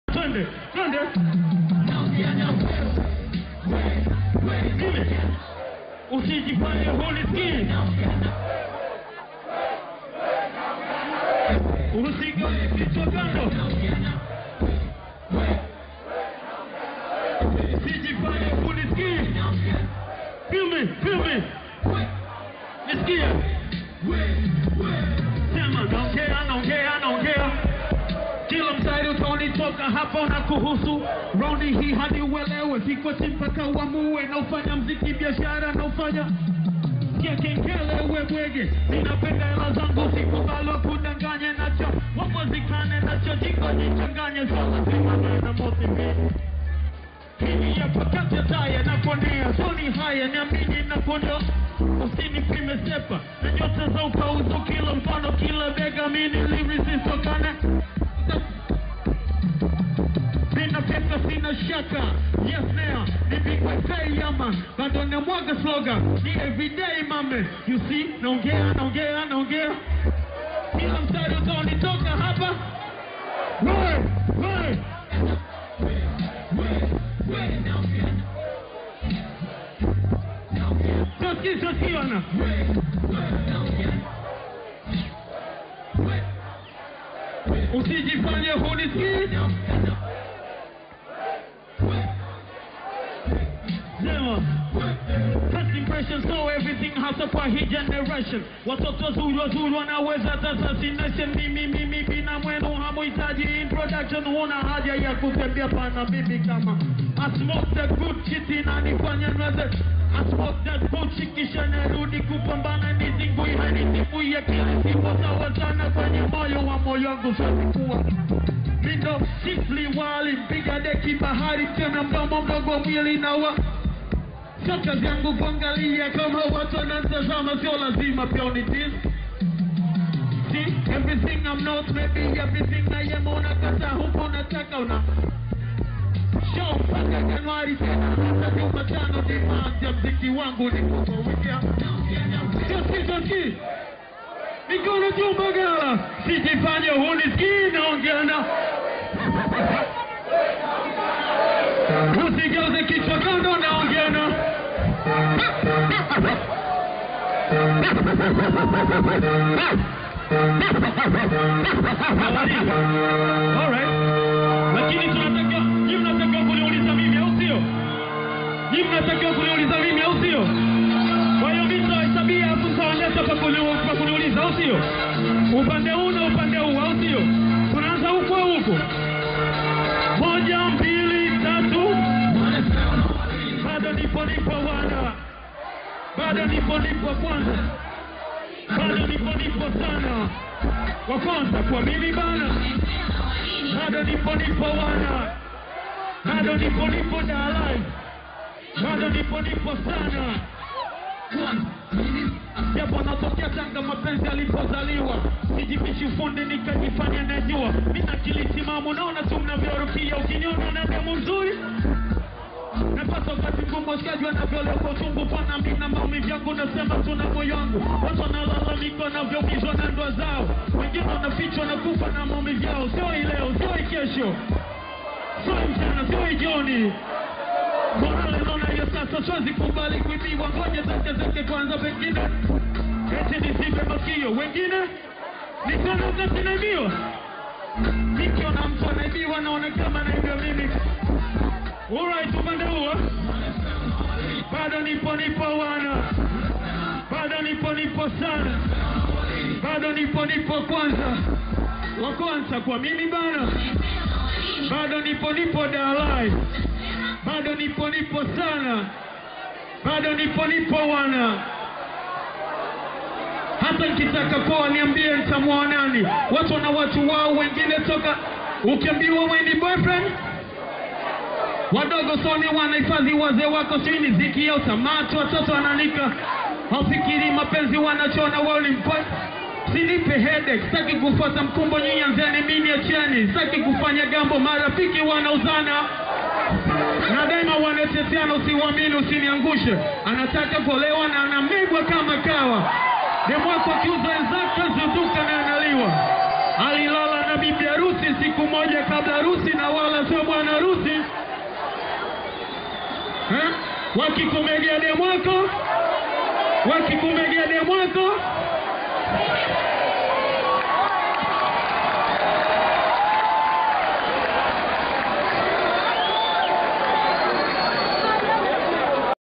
MWANAFA AKIWARUSHA MASHABIKI NDANI YA DAR LIVE
MwanaFA akiwarusha mashabiki waliofurika ndani ya ukumbi wa kisasa wa Dar Live Jumapili Juni 24, 2012 wakati wa shoo yake katika Tamasha la Wanaume Family Spesho.